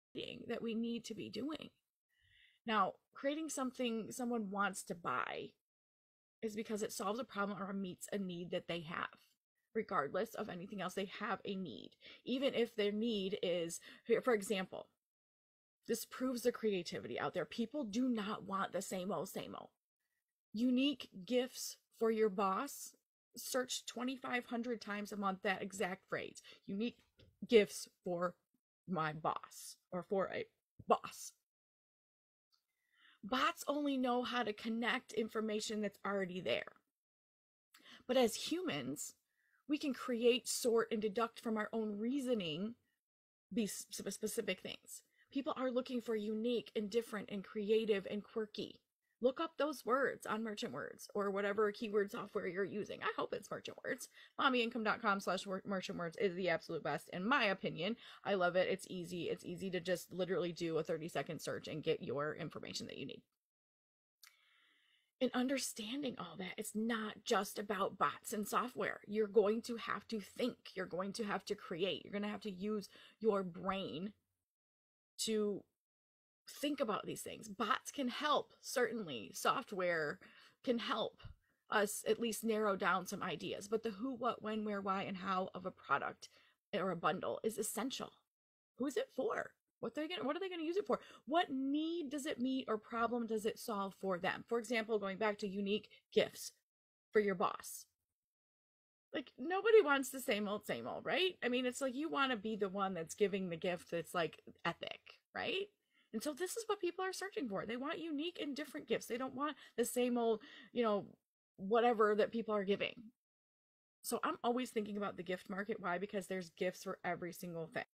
How much are you willing to spend on a gift for someone?
All the research show that the average gift for mostly anything, even birthdays, right now is around $30 to $60. And it depends on how close you are to someone. Usually, if you are closer, that number goes up for a spouse, a sister, a mother, and a dad. People are willing to spend more on people that they are closer to.

LEARN HOW TO BUNDLE NOW!